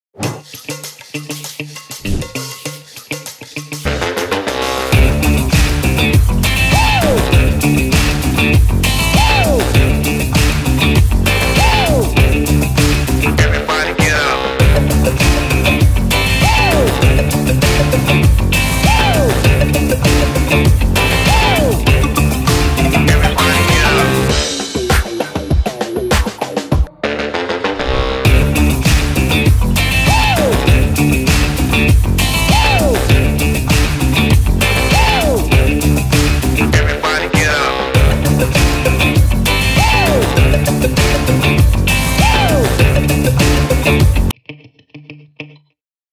V-Motion 180 Photo Booth: "90's Party" Output
A demonstration of a "90's Party" themed output created with V-Motion, the vertical 180 photo booth.